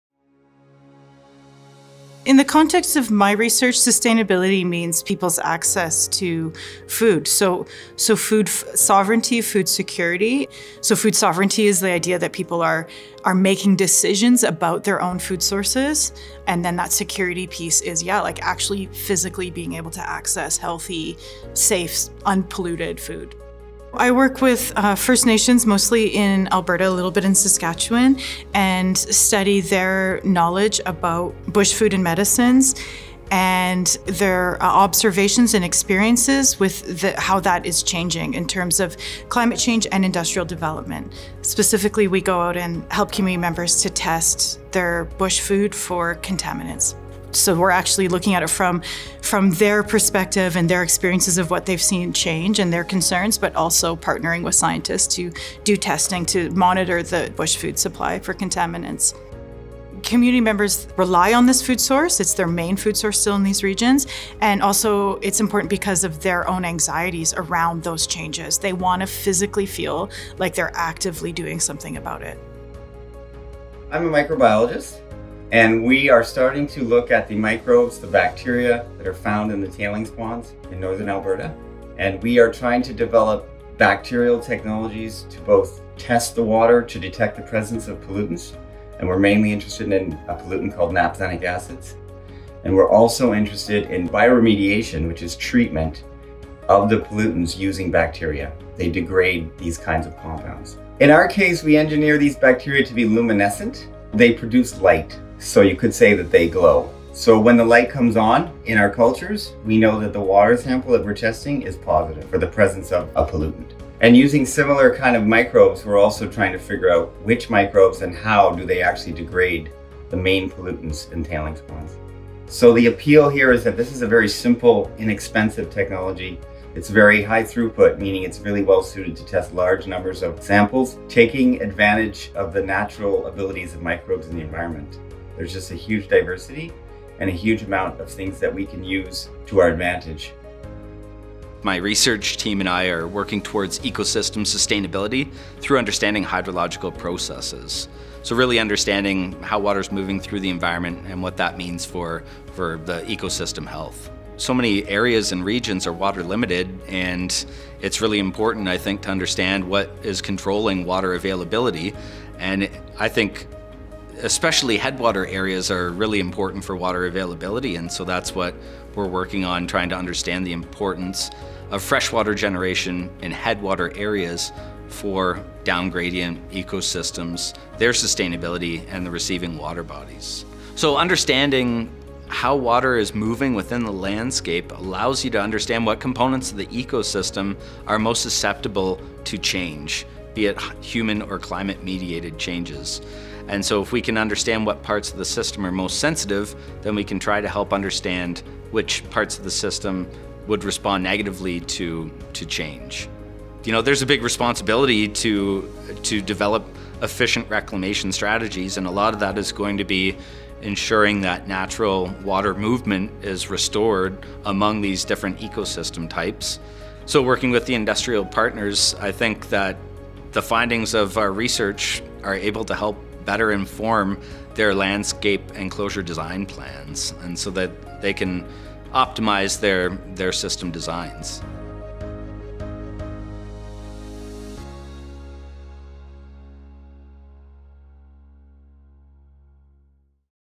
Supporting environmental sustainability
Sustainability is about finding ways to meet our needs for today and for the the future. For Indigenous communities in northern Alberta, it’s about protecting water and food sources that are under threat from industry so that future generations can practice traditional ways of living. It’s about understanding how environments and systems work, interact, and respond to emerging threats such as climate change. It’s about developing new technologies that can solve environmental problems at the local, regional, and global scale.